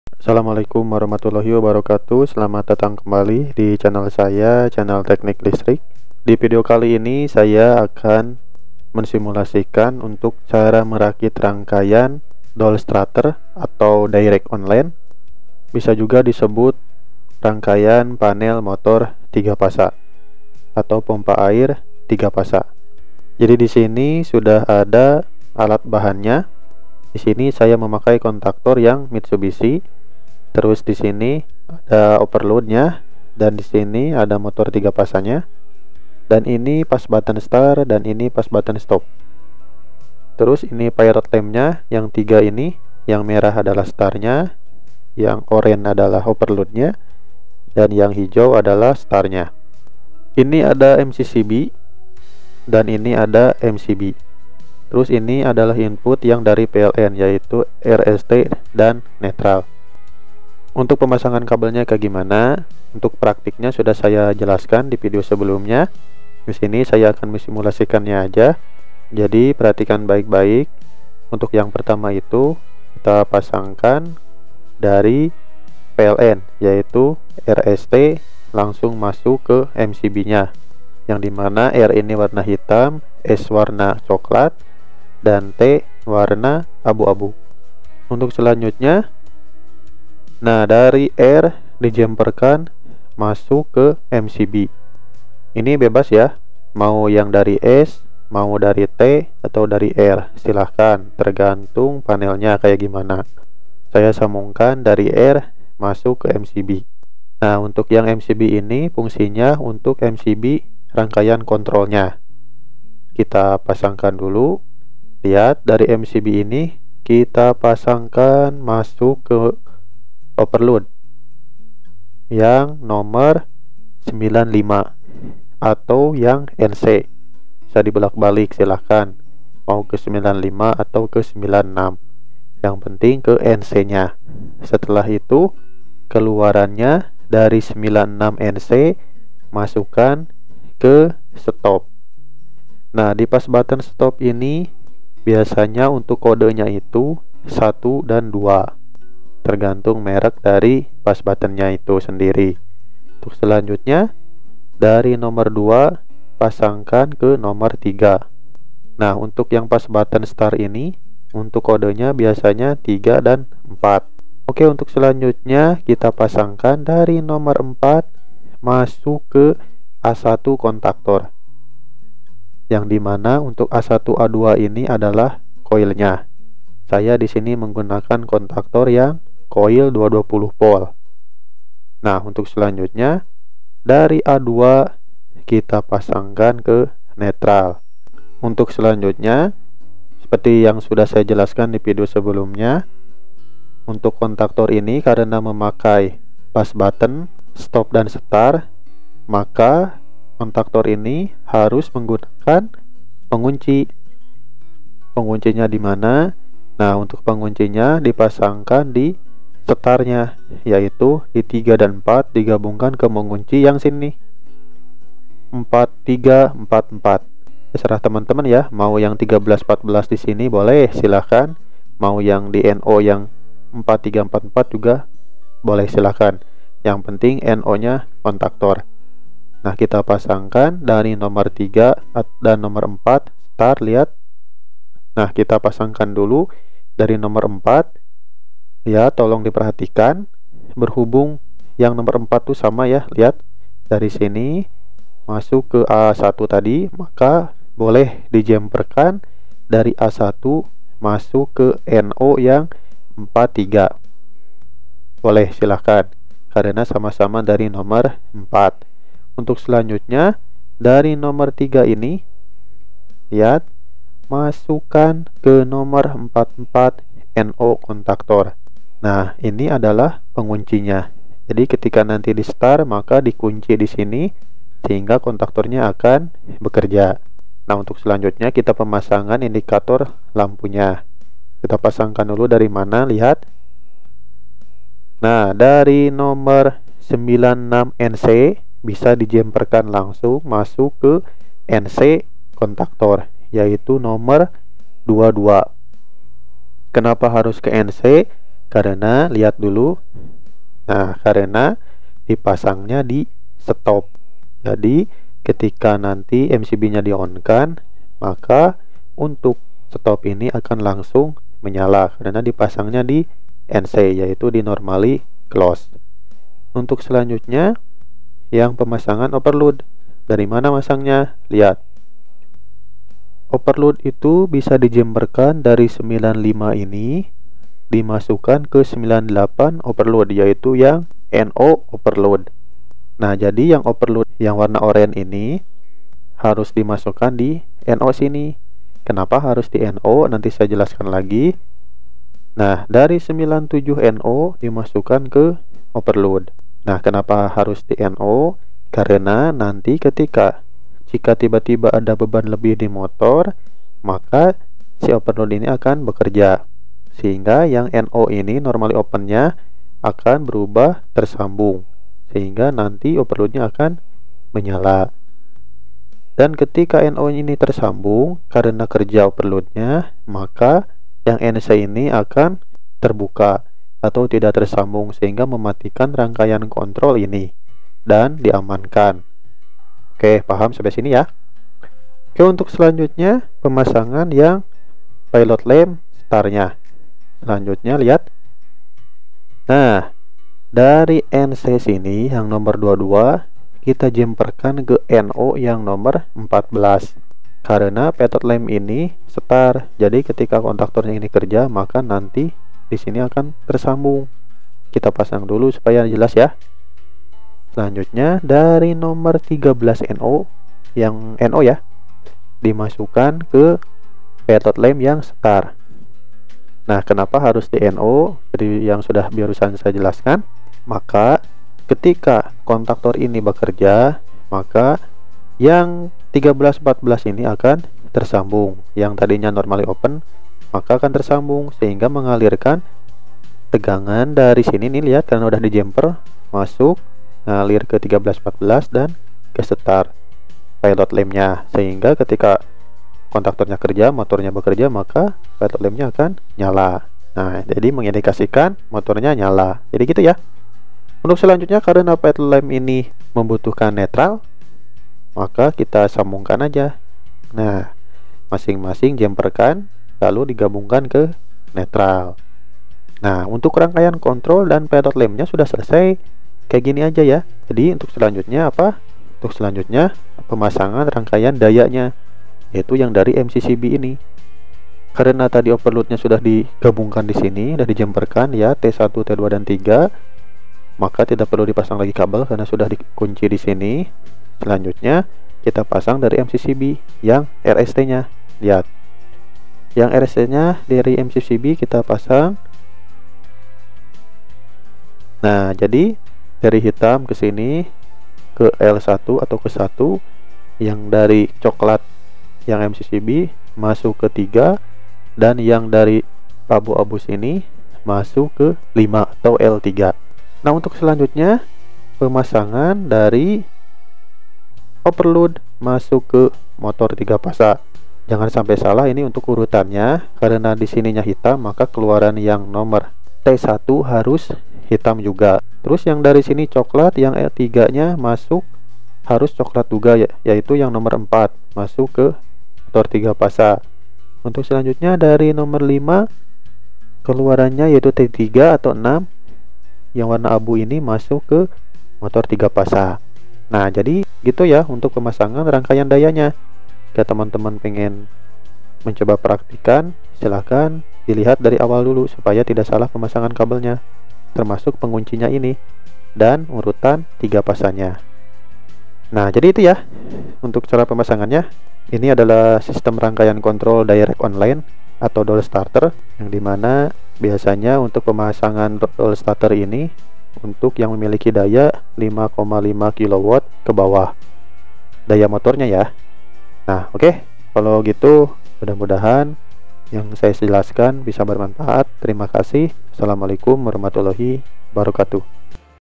Cara Merakit Rangkaian DOL Starter (Direct On Line) Motor 3 Phase
Maaf pilot lamp yg warna merah itu indikator STOP.

Cara Merakit Rangkaian DOL Starter (Direct On Line) Motor 3 Phase ini sudah sering di bahas saat sekolah SMK Listrik. kali ini saya akan mensimulasikan cara pasang kabel nya kaya gimana.
untuk praktik nya tonton video ini :

alat bahan :

- Kontaktor coil 220 V
- Overload
- MCB
- MCCB / MCB 3 phase
- Push button start stop
- Motor 3 phase
- Pilot lamp